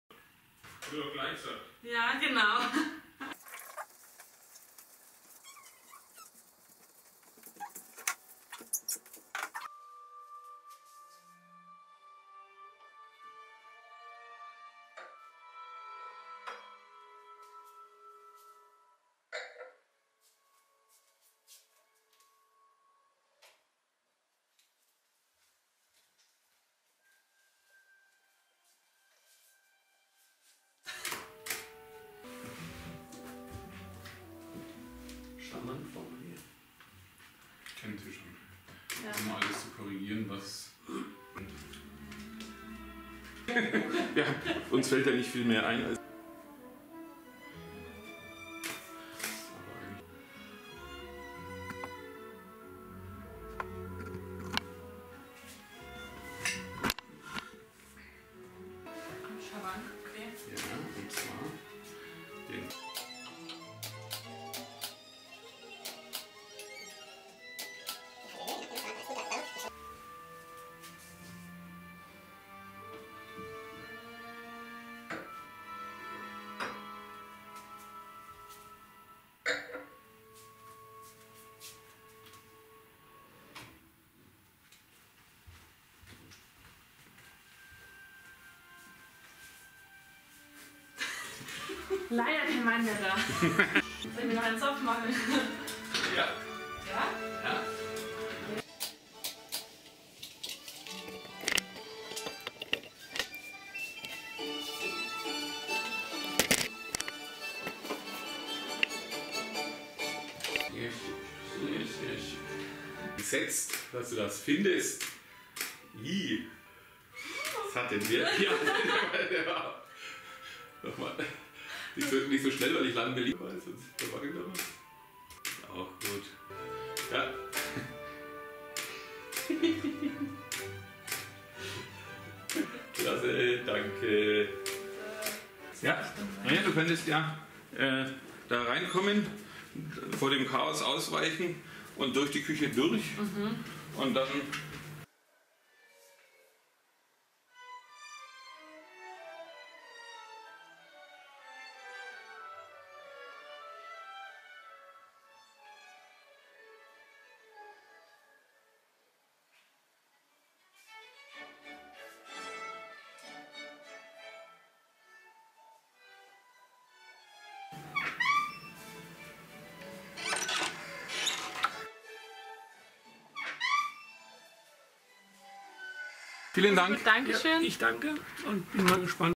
Kompletteinrichtung in Kersbach Teil 2: DAS WOHN-, ESS- UND ARBEITSZIMMER
Kompletteinrichtung mit Massivholz und "alles Gute zum Einrichten" vom Fußboden bis zur Beleuchtung am Beispiel Kersbach:
00:00 erstmal Aufbauen und Aufräumen
00:09 360°-Ansicht mit Panono
00:25  Wohnzimmer und Essplatz
01:02 Lifttür im Glas-Holzdesign
01:31 Arbeitszimmer und Zopf
02:15 Rundgang Küche-Arbeitszimmer-Essplatz-Wohnzimmer
02:51 Abbauen und Einladen